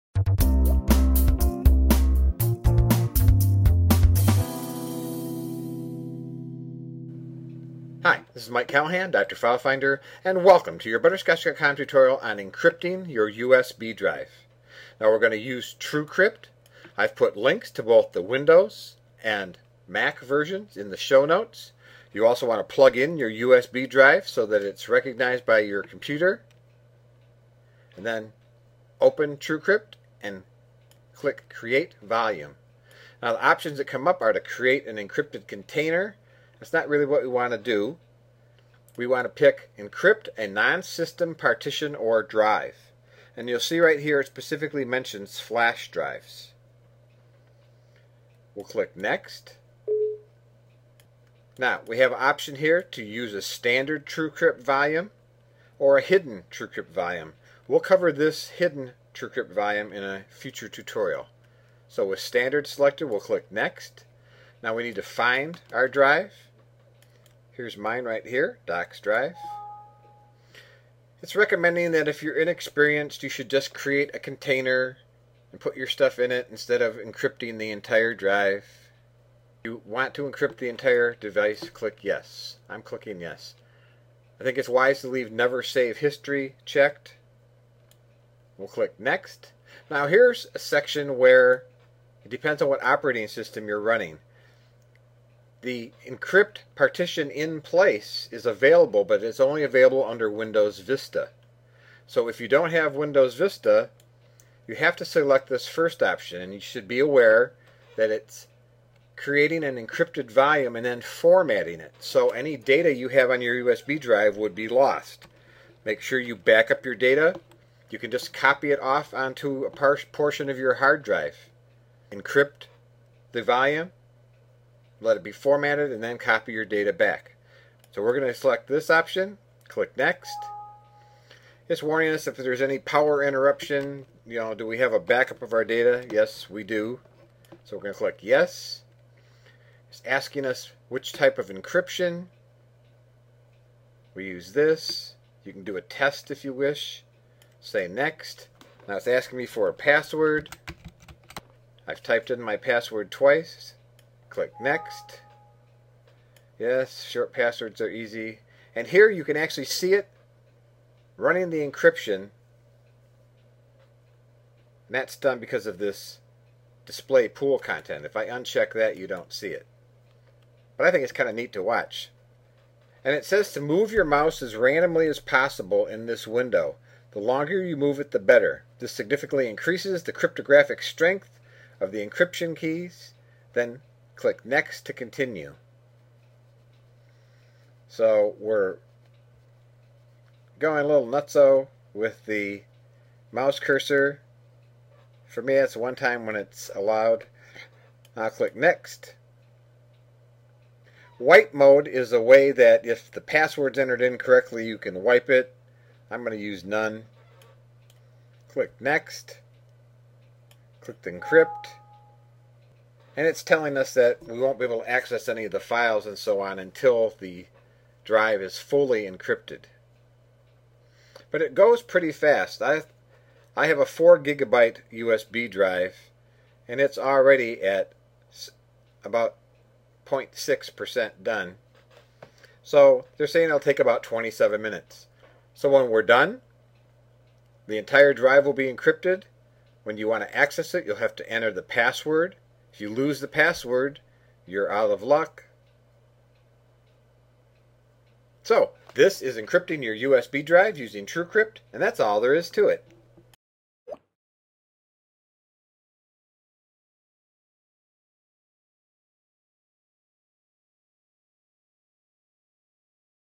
How to encrypt a USB key with TrueCrypt
If you have a
USB drive
you can carry quite a bit of information around on it. Documents, passwords, bookmarks, and more. If you have something like
PortableApps
installed on it you could have even more information. It might be wise to
encrypt your USB drive
just in case you lose it.  A Internet tutorial by butterscotch.com.